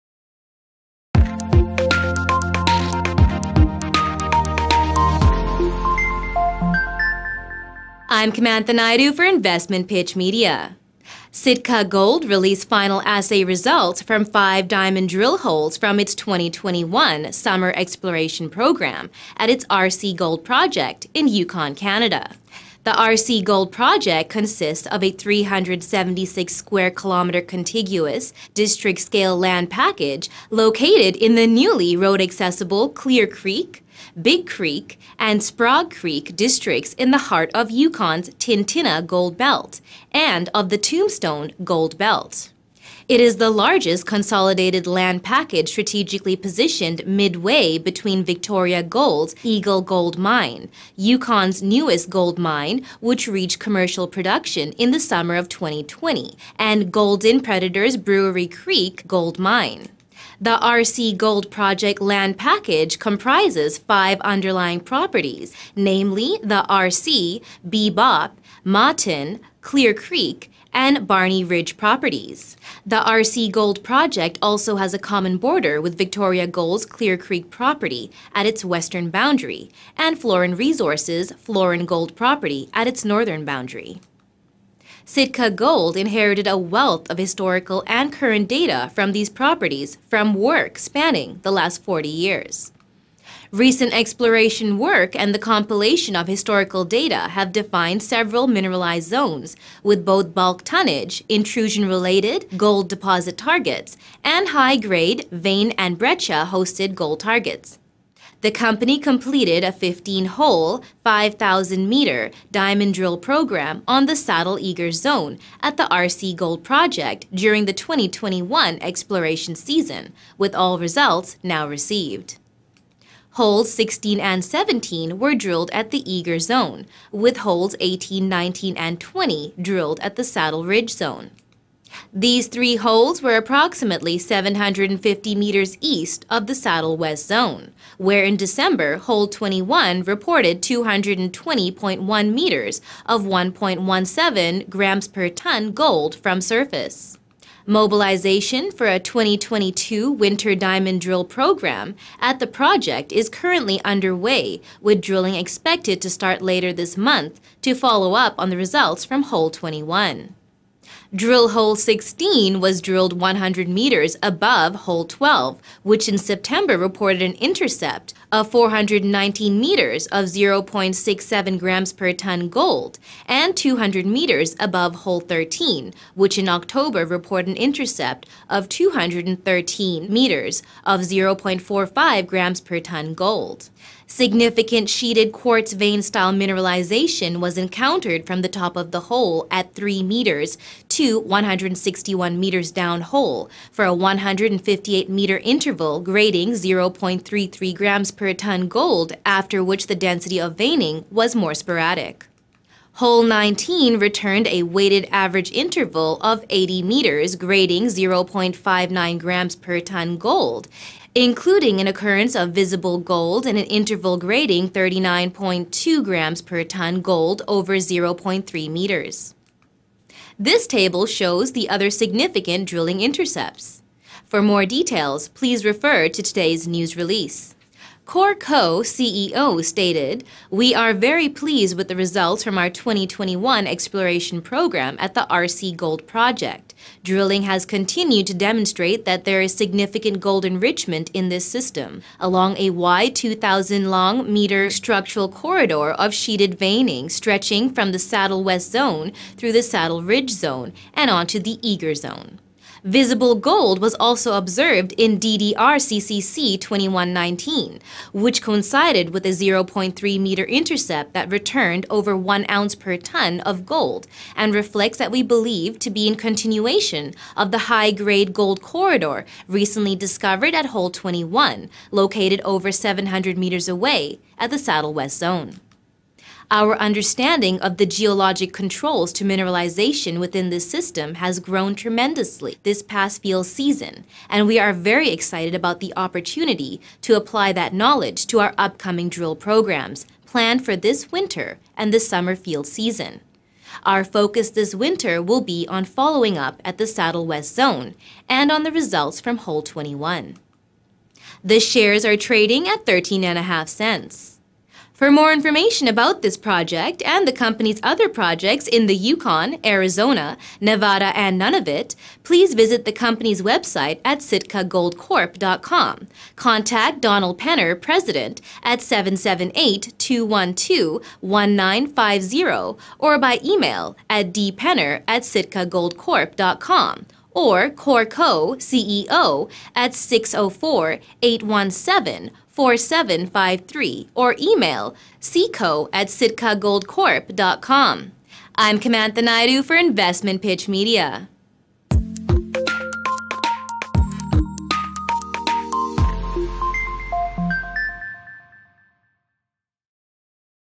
Sitka Gold reports 16.0 metres of 2.18 g/t gold at RC Gold in Yukon
Sitka Gold released final assay results from 5 diamond drill holes from the its 2021 summer exploration program at its RC Gold Project in Yukon, Canada.